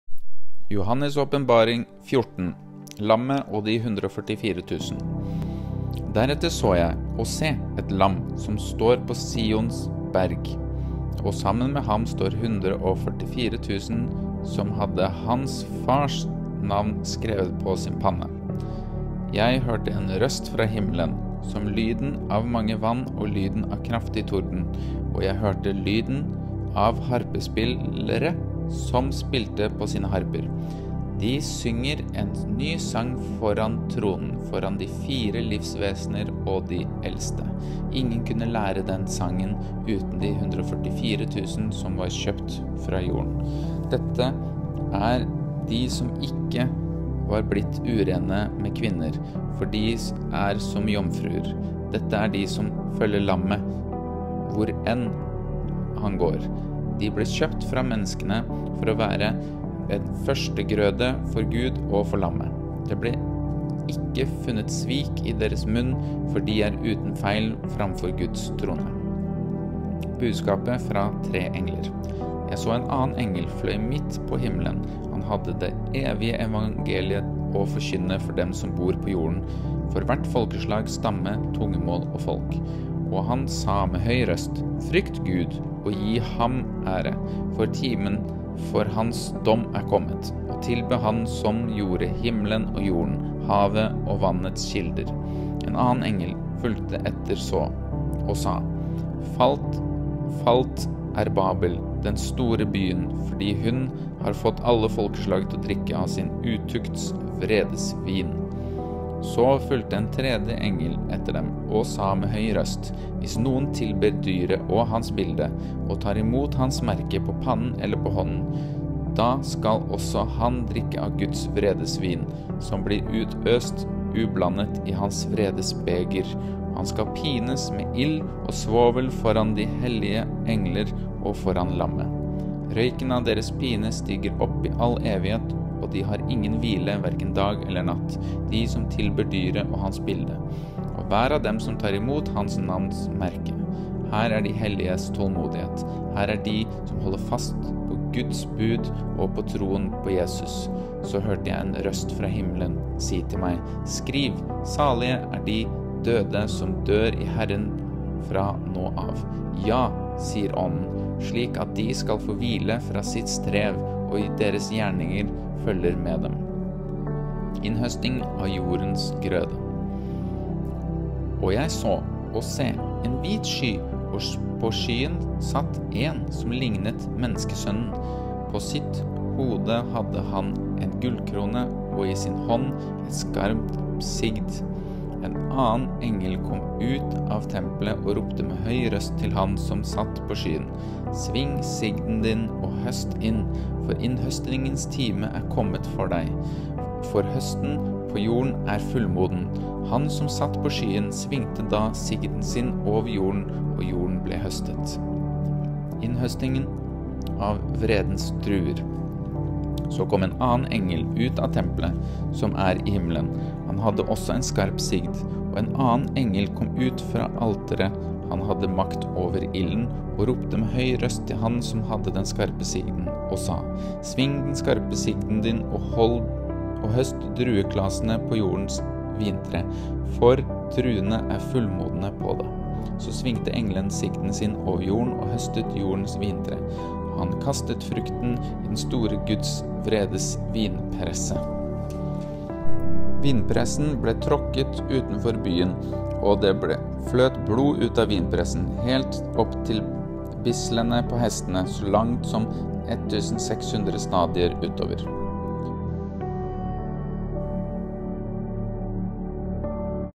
Johannes Åpenbaring 14 - Lammet og de 144 000
Johannes’ åpenbaring 14 Lammet og de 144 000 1 Deretter så jeg, og se, et Lam står på Sions berg, og sammen med Ham 144 000 som hadde Hans Fars navn skrevet på sin panne. 2

For så høyt elsker Gud oss at Jesus ble uskyldig ofret og påtok seg all verdens synd og sykdom slik at vi kan ha liv.
Jesus Kristus er veien, sannheten, livet.

Johannes' åpenbaring (eller apokalypsen) er det siste skriftet i Det nye testamente, den andre delen av kristendommens hellige skrift Bibelen. Teksten er en apokalypse, en fortelling om endetiden.